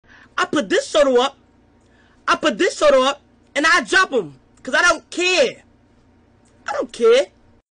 Cameron Brink TikTok’n with the Curry kids
Stanford forward Cameron Brink TikTok’n with the Steph Curry kids. 9/14/2021